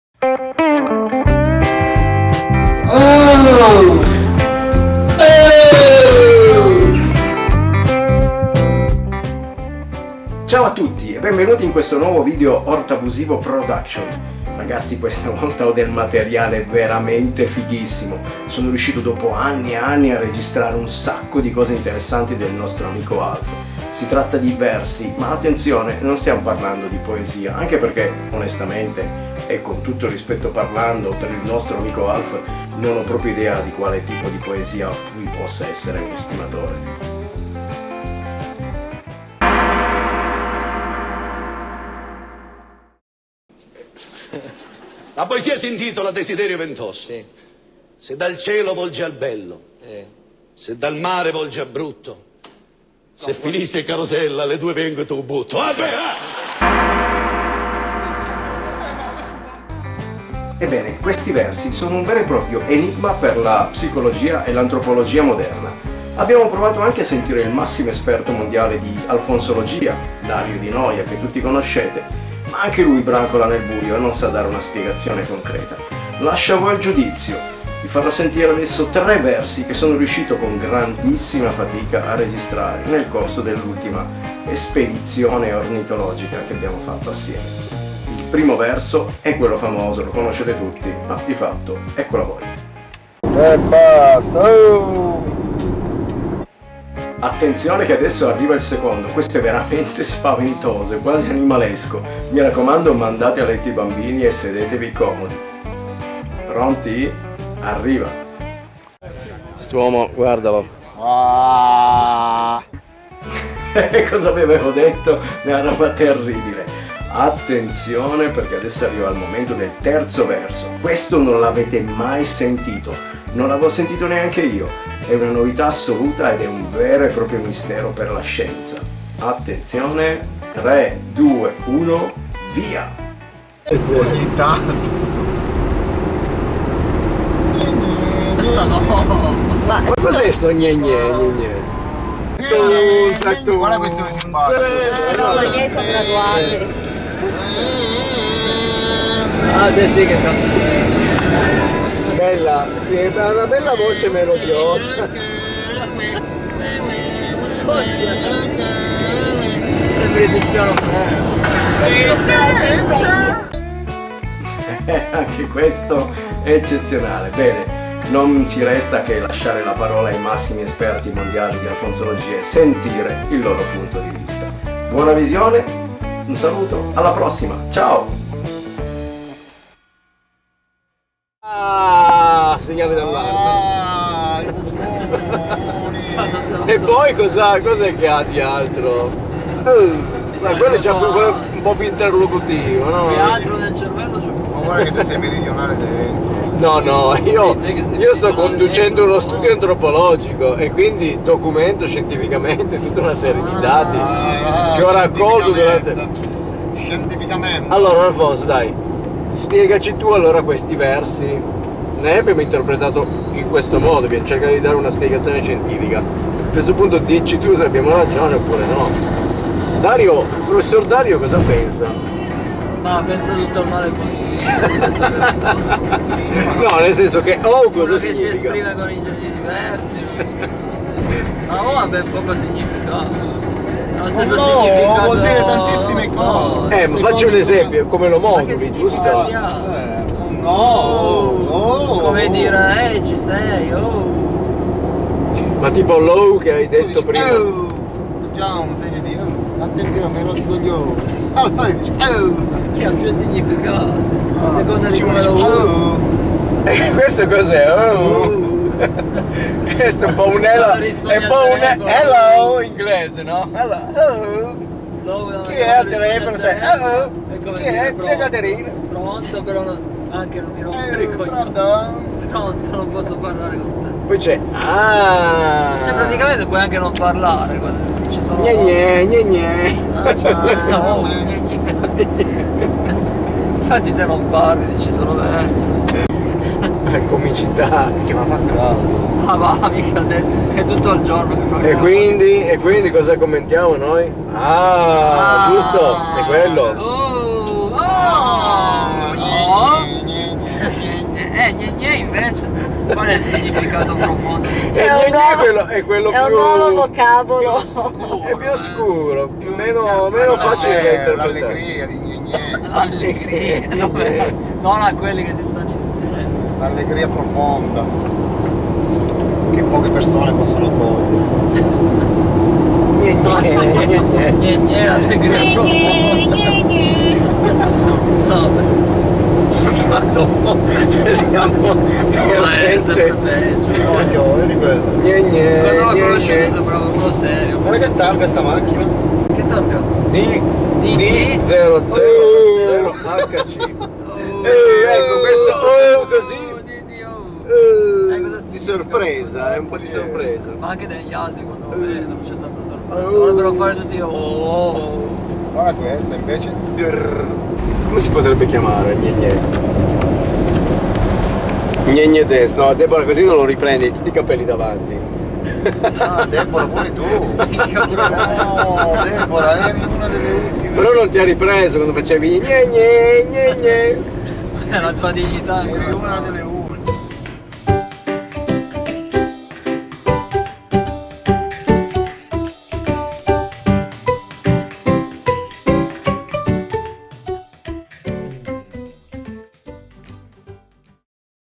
Alfonzo in versi...
Esprimersi in versi. ehm, esprimersi coi VERSI, è una prerogativa di Alf, il quale non perde occasione per sfoggiare una nuova performance con amici e sconosciuti vari.
I prodi Dario e Roberto provano a interpretare scientificamente la vasta gamma di espressioni vocali prodotte dal nostro simpatico soggetto...